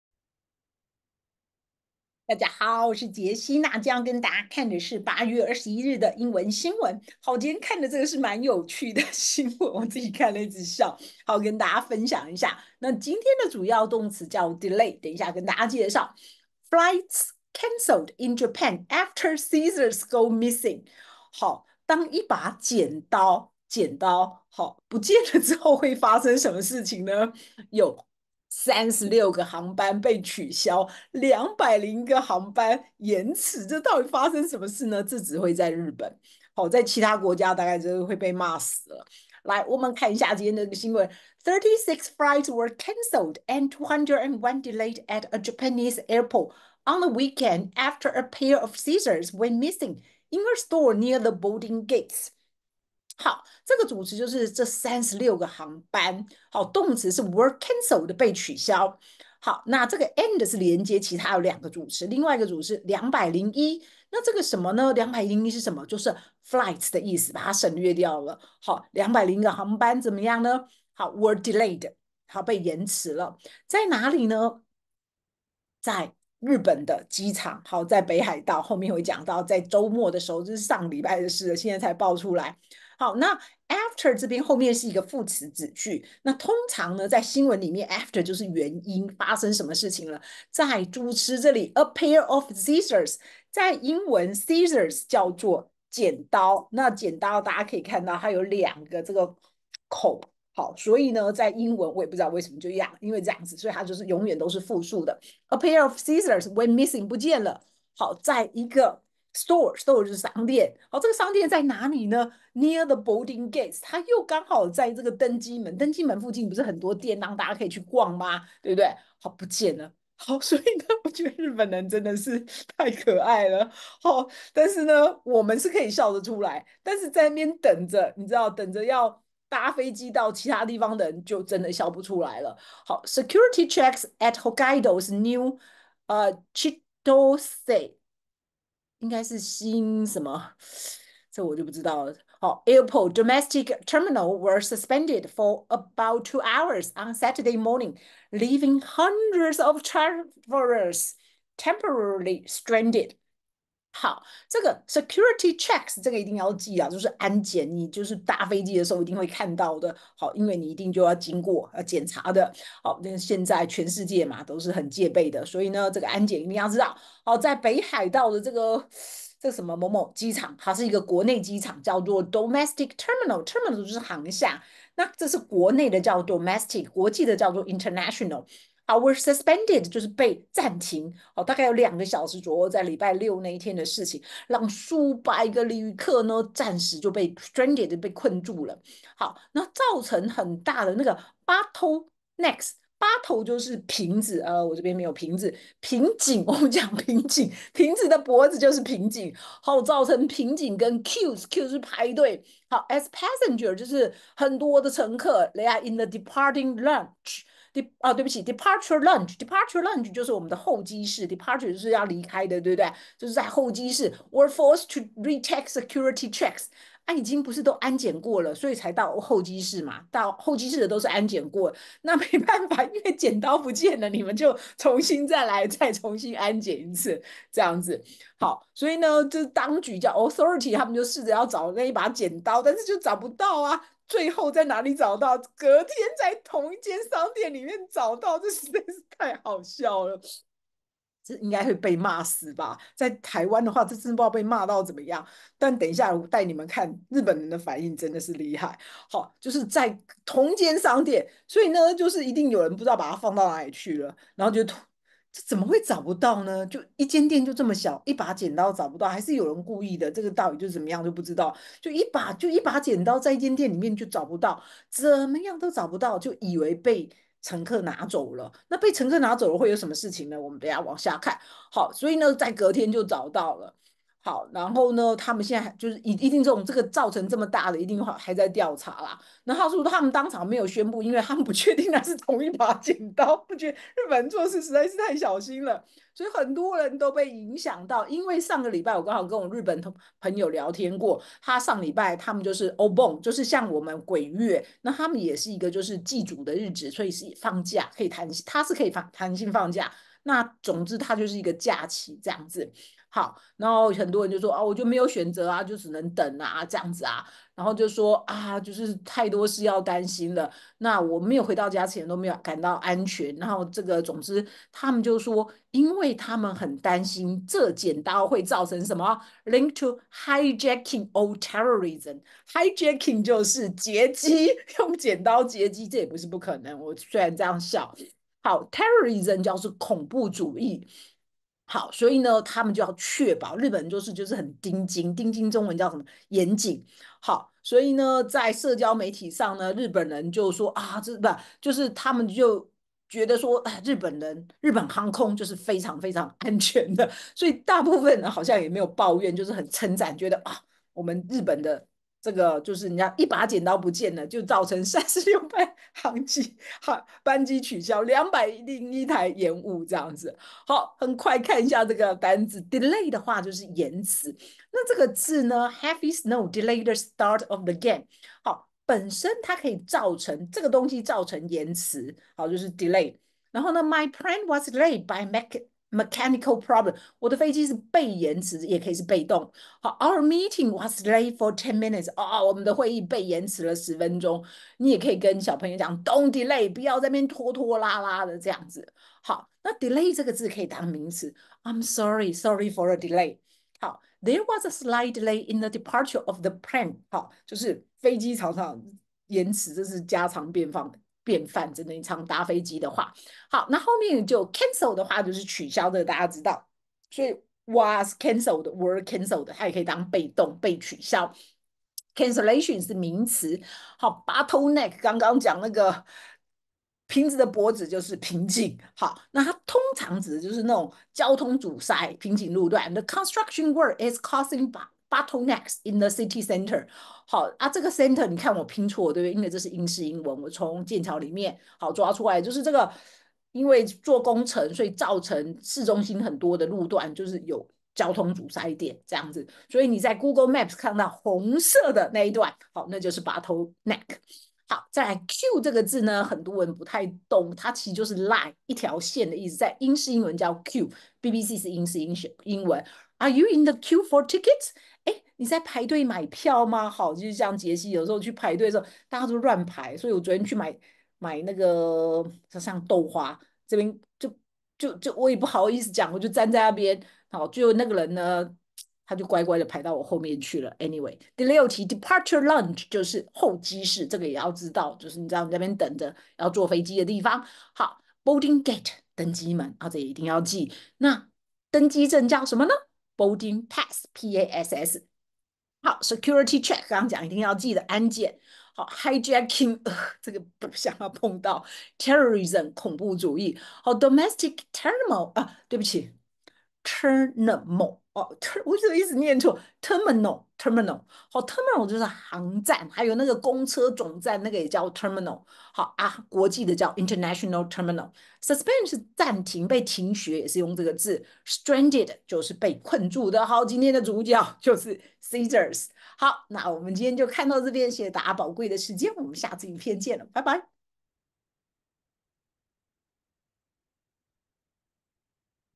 【潔西家】2024/8/21 新聞單字 delay 與機場相關單字 在日本機場剪刀不見時會發生什麼事呢？
部落格文字檔連結：